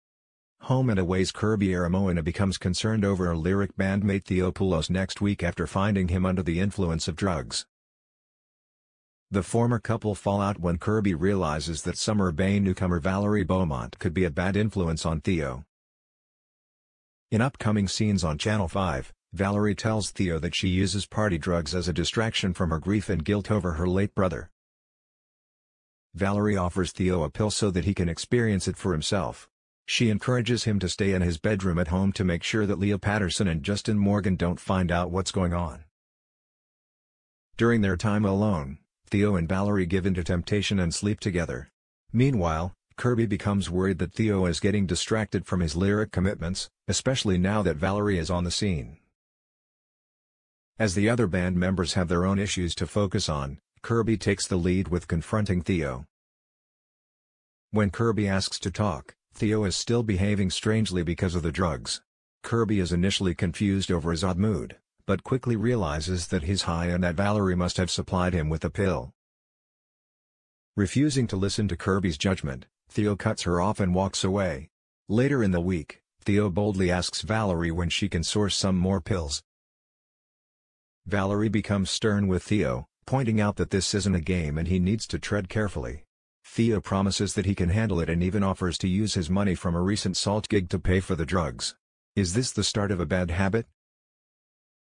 Home and Away Spoilers: Former couples clash and the dark story of Valerie and Theo
Home and Away: Former couples clash and the dark story of Valerie and Theo...